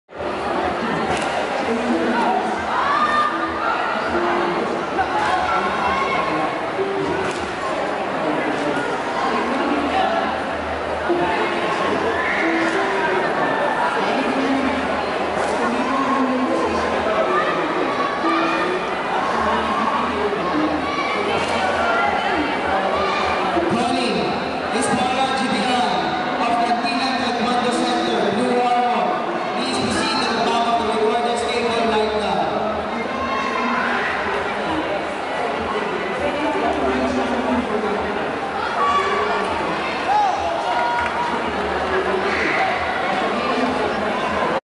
2018 CARAGA AGE GROUP TAEKWONDO BUTUAN - JAZMINE KAYE AMORES OF MADRIG
Jasmine Kaye Amores of Madrig Taekwondo performs.